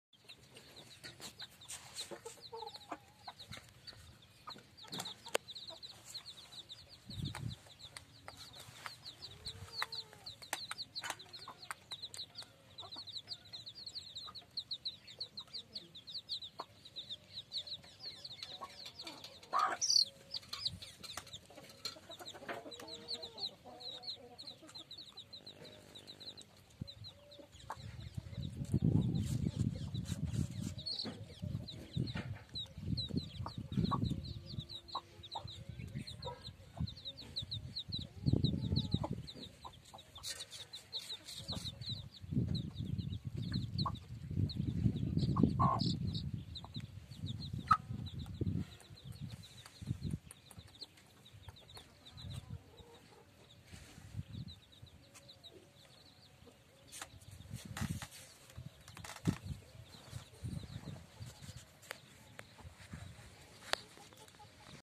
Kadaknath Murga with Desi Murgi | Kadaknath Ke Chuje
Kadaknath Murga with Desi Murgi | Kadaknath Ke Chuje kadaknath and family eating food with chicks and hen kadaknath ke bacche aur desi murgi ke bacche bache